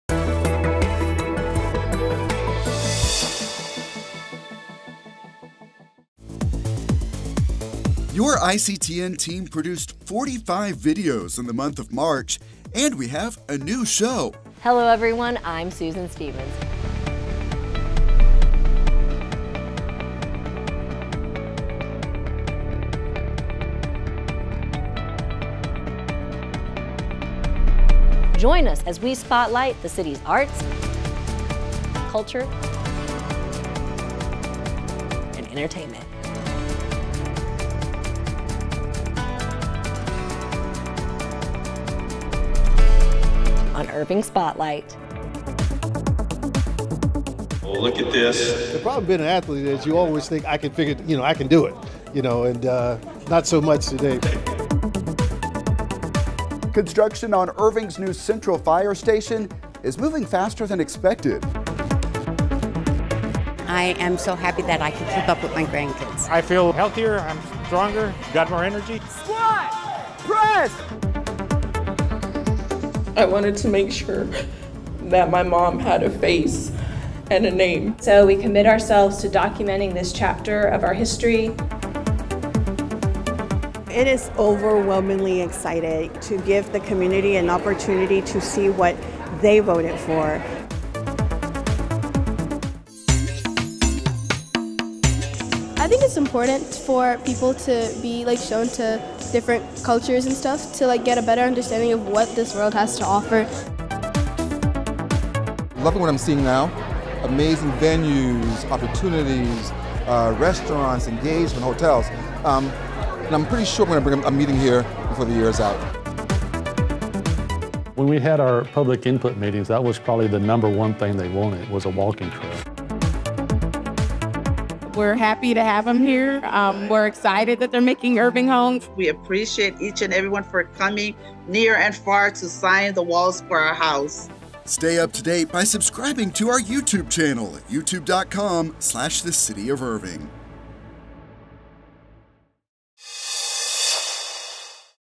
Last Month on ICTN - March 2024
From the amazing teen talent in Irving, to the active seniors in a moving exercise class, to an update on the downtown fire station, we love to tell Irving's stories - see what we've been up to.  Plus, we have a great new show on arts, culture, and entertainment in the city - Irving Spotlight.